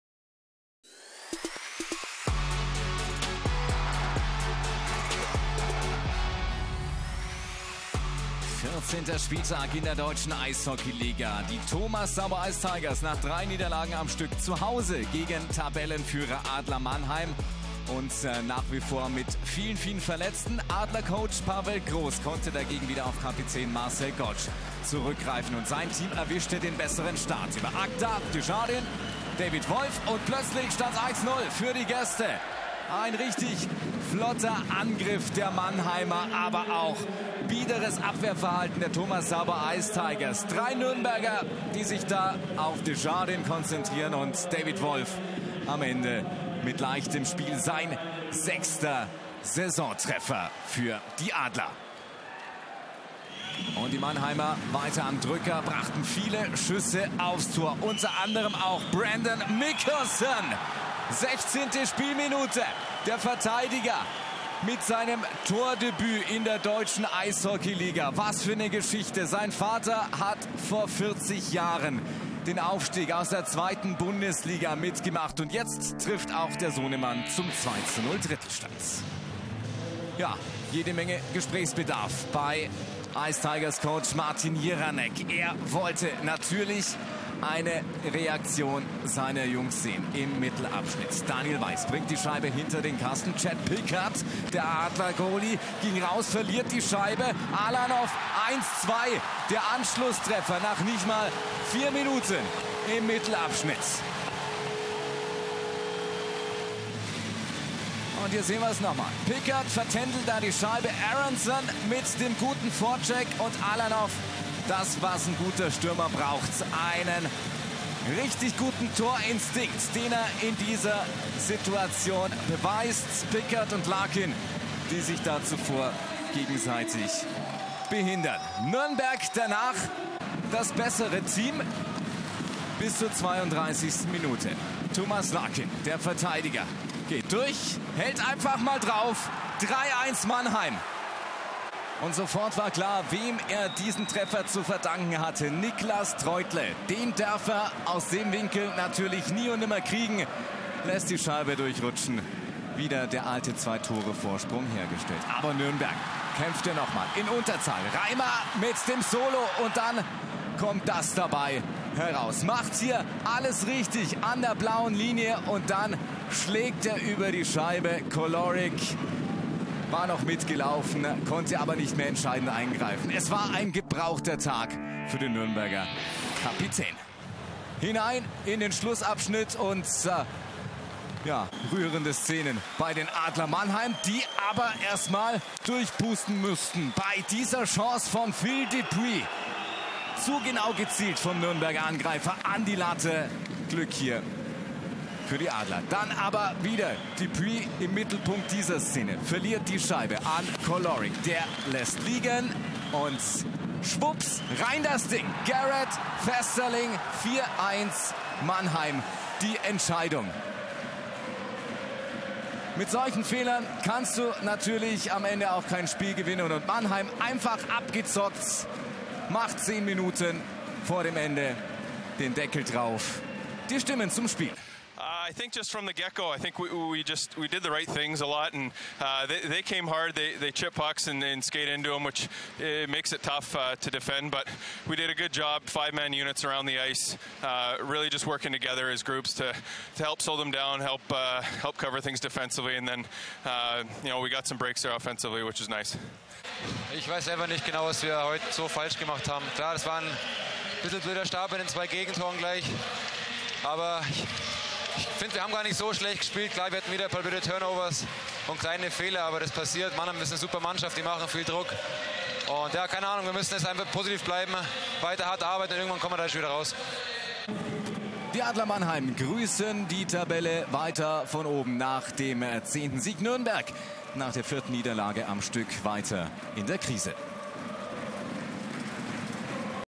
Thomas Sabo Ice Tigers - Adler Mannheim | 14. Spieltag, 18/19 | Telekom Sport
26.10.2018, 14. Spieltag der Saison 2018/2019. Der Tabellenführer siegt weiter!
Die Adler Mannheim bezwingen die Thomas Sabo Ice Tigers 4:1. Den bessern Beginn erwischten die Gäste aus Mannheim, die nach fünf Minuten durch David Wolf in Führung gingen. Brendan Mikkelson steuerte das 0:2 aus Sicht der Gastgeber bei (16.). Für einen Hoffnungsschimmer sorgte Eugen Alanov, der mit seinem Treffer den Anschluss markierte (24.). Die Adler Mannheim sorgten allerdings durch Thomas Larkin (32.) und Garrett Festerling (50.) für die Entscheidung. Für die Thomas Sabo Ice Tigers ist es die vierte Niederlage in Folge.

Aufstellungen:

Thomas Sabo Ice Tigers:
Brandon Buck, Jason Bast, Christopher Brown, Tim Bender, Taylor Aronson, Niklas Treutle

Adler Mannheim:
Benjamin Smith, Tommi Huhtala, Markus Eisenschmid, Brendan Mikkelson, Sinan Akdag, Chet Pickard